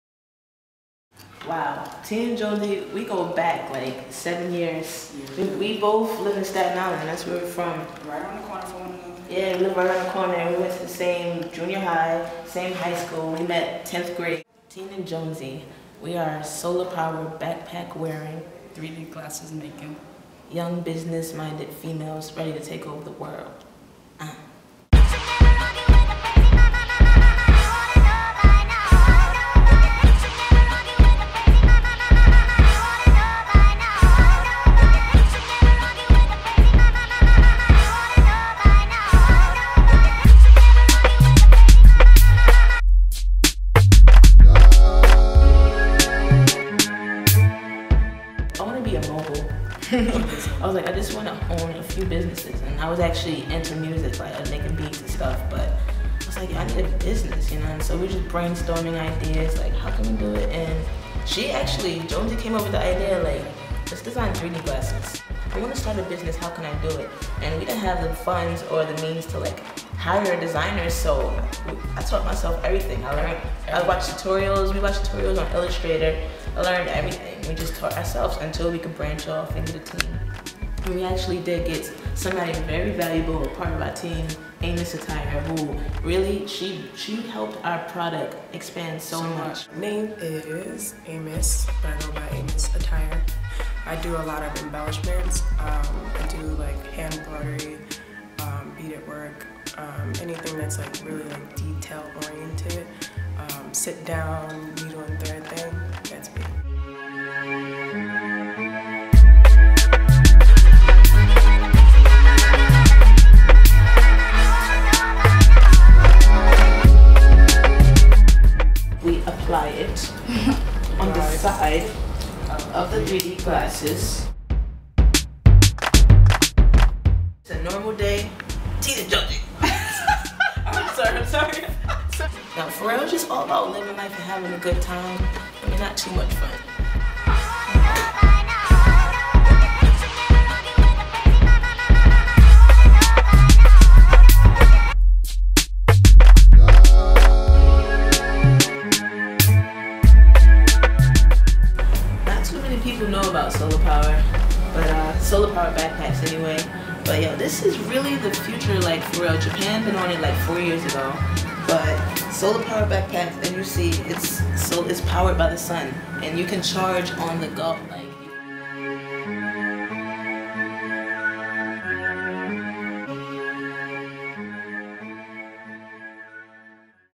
Teen+Jonesy
Here is a re-cap of teen&jonesy during their freshman year (summer collection 2010).